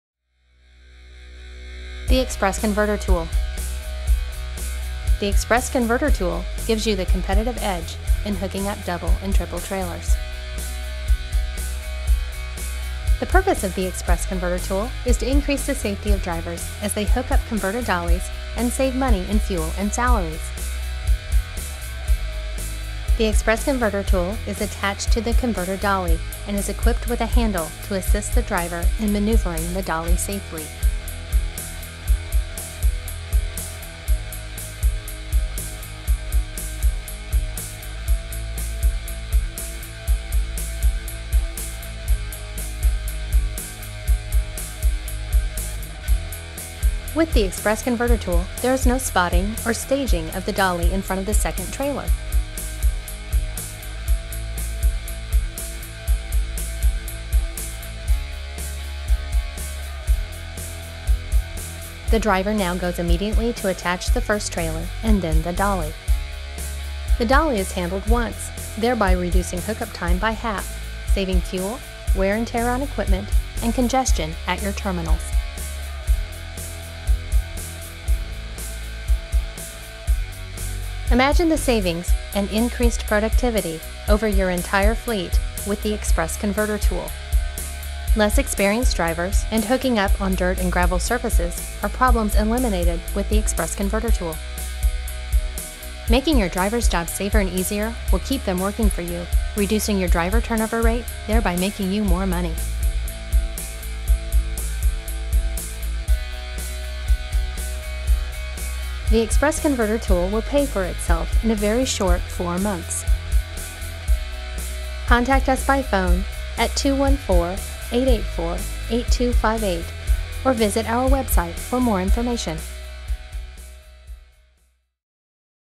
X-Press Converter Tool In Action! Find out more at xpressconvertertool.com.
THE X-PRESS CONVERTER TOOL stabilizes the converter dolly thereby providing drivers with full control of the dolly as they connect to the next trailer.

IMMEDIATE BENEFITS ARE:
- product pays for itself in 3-4 months with 2 uses per day
- guaranteed savings of 50% connection time for the best drivers
- guaranteed savings of 75%+ with tandem dollies and a 1 person connection process
- drivers are driving, not climbing in/out of cab & fighting dolly to connect

HOW & WHY:
Many drivers encounter issues when hooking up double and triple trailers.  Injuries, multiple trips in and out of the cab, uneven surfaces, inexperience, and poor weather conditions are just a few.  The patented X-Press Converter Tool provides the solution to these problems.

The X-Press Converter Tool eliminates the need to stage the converter dolly. The dolly is only handled once, thereby reducing hook up time by half.  By reducing hook-up time, the XCT reduces transit time, getting your goods to their destination quickly.  The XCT maximizes the utilization of every trailer for every load picked up and dropped off.